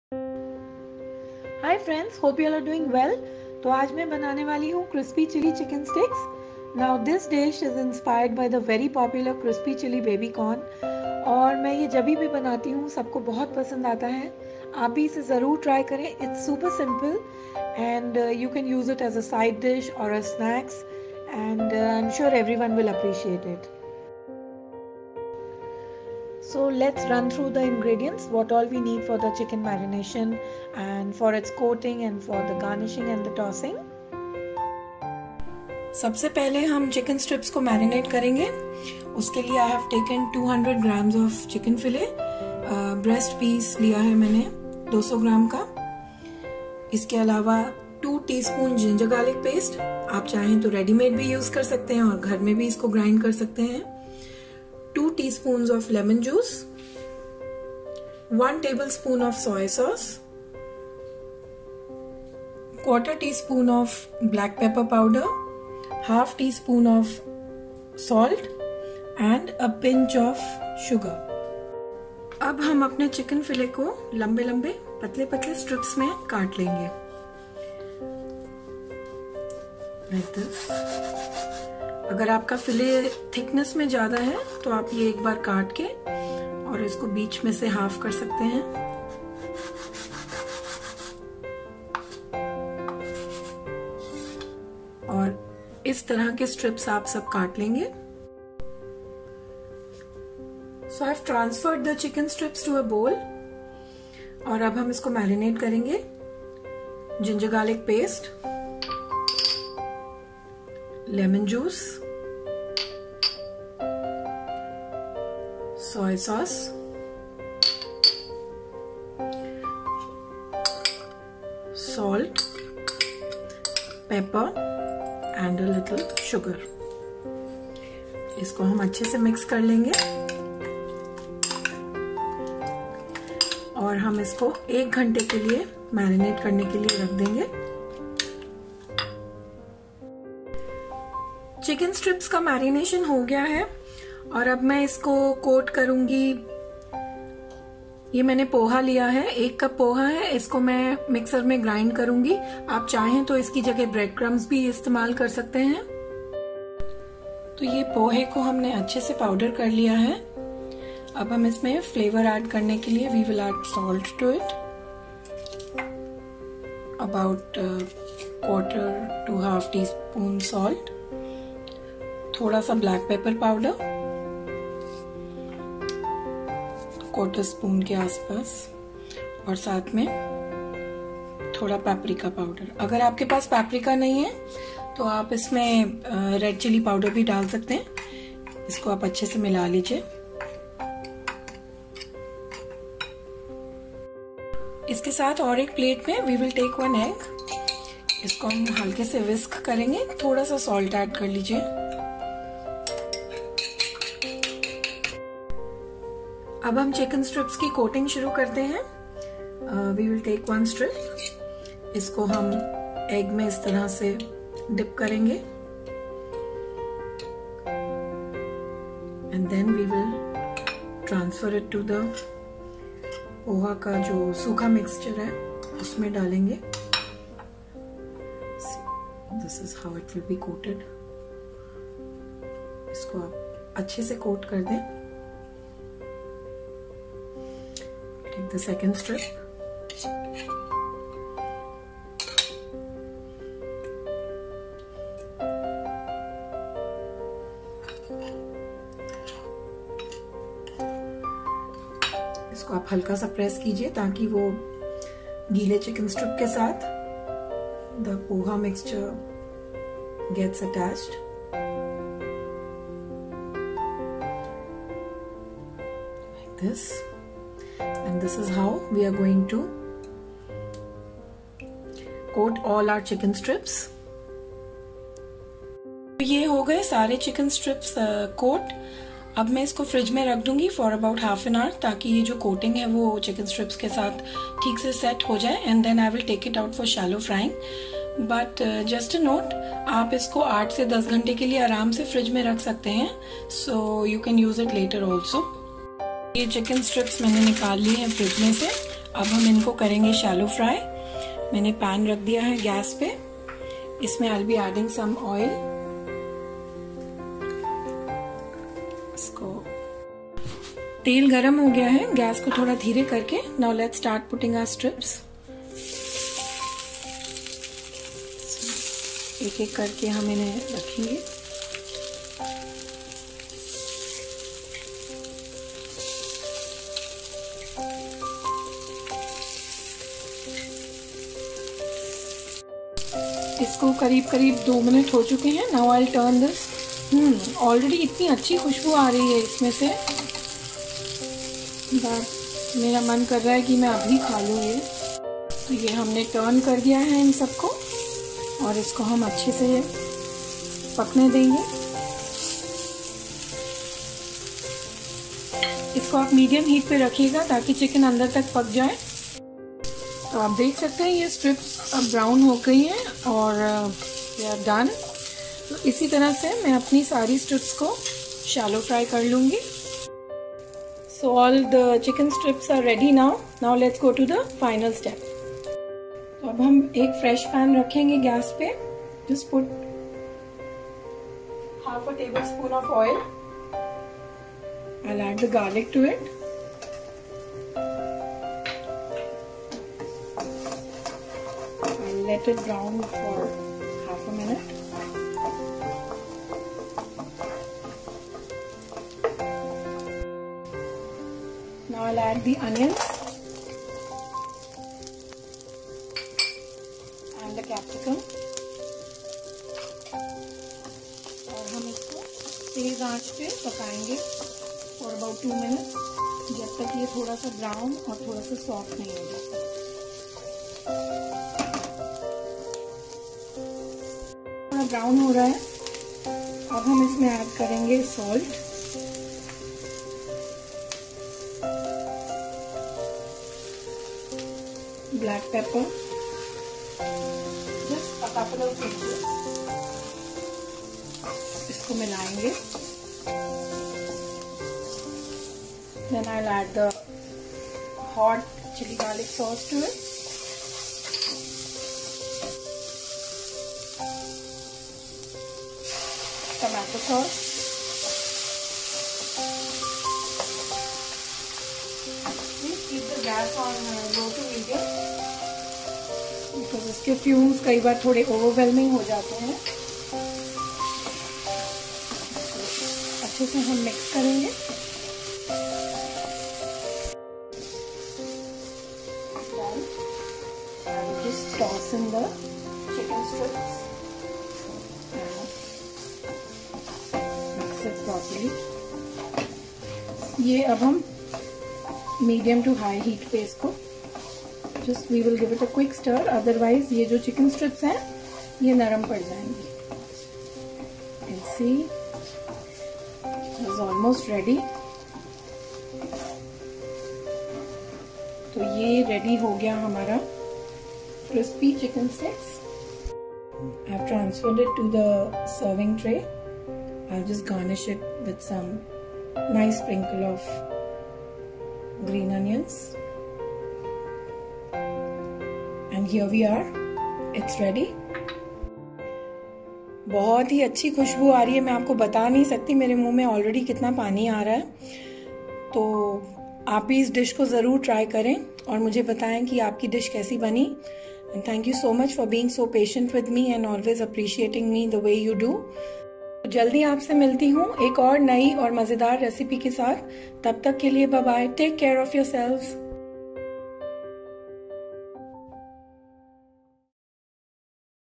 Crispy Chilly Chicken Sticks-A Delicious Indian/Desi Chinese Appetizer/Side Dish
Crispy Chilly Chicken Sticks is inspired by the extremely popular CrispyChillyBabyCorn . It is an ideal Appetizer or a Side Dish with Rice or Noodles, made with juicy tender Chicken strips coated and made crispy with the very healthy flattened rice and air or pan-fried (weighing less on your guilt for the fried ) and tossed in a succulent blend of tantalizing Chinese cuisine inspired sauces. This dish may easily become a perfect contender for your most popular party menu snacks . It is a must-try with its super simple recipe , crunchy texture and delectable flavor.

A ready reckoner of all the ingredients to give you a head start :

Chicken Marination :
Chicken Boneless        200gms(If using frozen, cut the strips before it is
                                        defrosted fully)
Ginger Garlic Paste     2 Tsp (Readymade or Homemade)
Lemon Juice                 2 Tsp
Soy Sauce (Dark)         1 Tbsp
Sugar                              A pinch

For Chicken Coating
Poha (Flattened Rice)          1 Cup (Powdered in dry grinder)
Or Bread Crumbs                  1 Cup
Paprika/Red Chilly Powder   ¼ Tsp
Egg                                           1

 For Tossing
Garlic                    4 Cloves (Finely chopped)
Onion                    1 Medium (Diced)
Capsicum             1 Medium (Diced)
Tomato Sauce      1 ½ Tbsp (Any Brand)
Soy Sauce Black  1 Tbsp
Hot Chilly Garlic paste   ½ Tsp (Homemade  or any brand available)

You can make your own Hot Chilly Garlic Paste. It can last for 2 to 3 months in the refrigerator and is a key ingredient to many Indian, Chinese or Mexican dishes to give that extra chilly kick to your palate .

For Garnish
Green Onions             1 Tbsp Chopped

Try this mouth-watering appetizer and you are bound to win some admirers for your culinary skills.